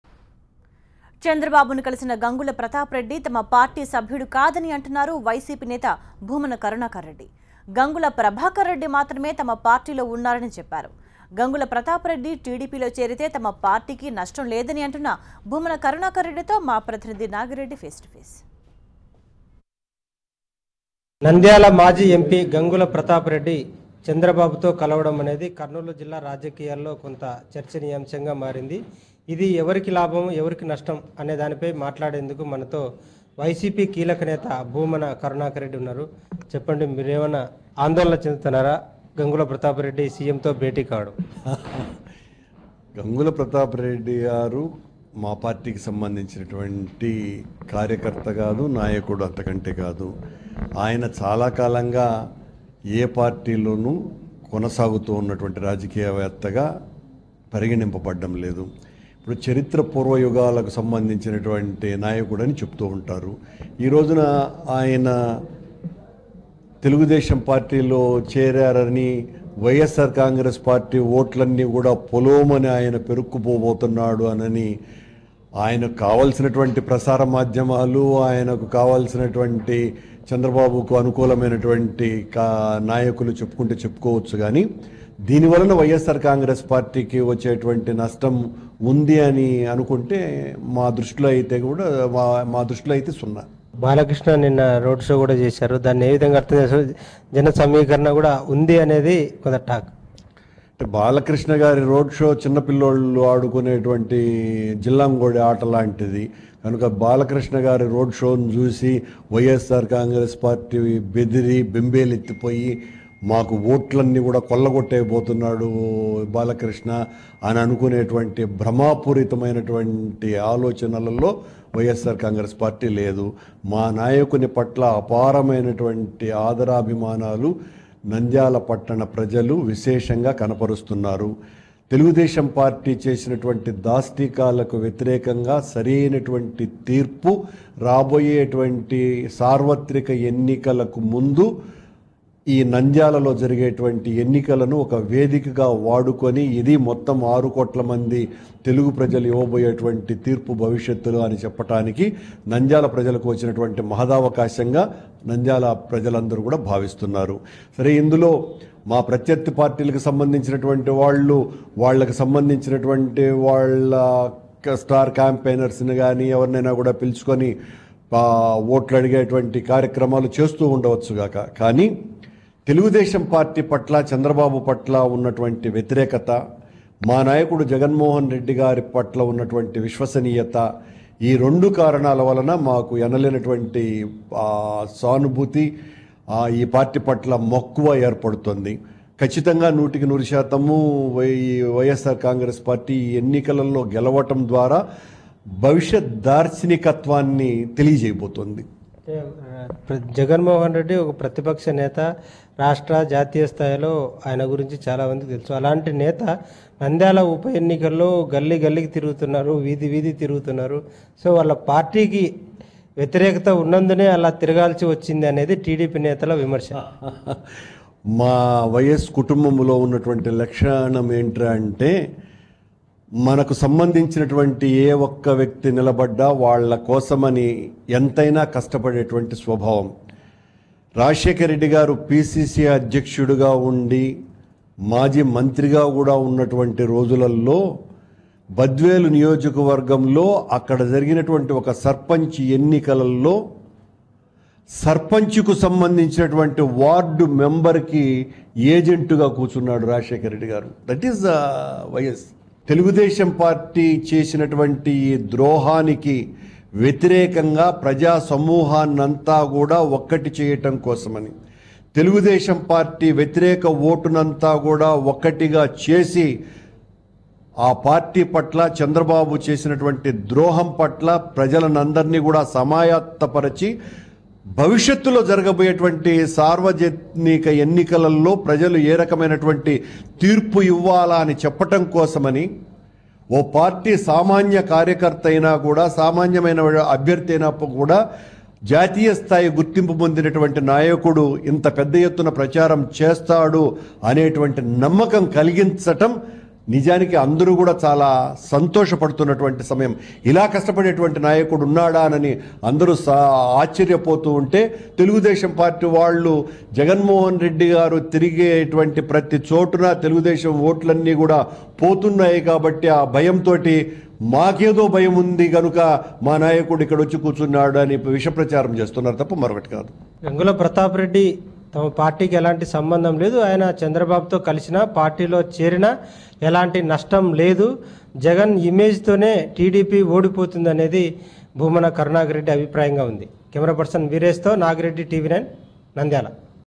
YCP loses nothing with Gangula Pratap Reddy's exit - Karunakar Reddy - TV9
YCP loses nothing with Gangula Pratap Reddy's exit - Karunakar Reddy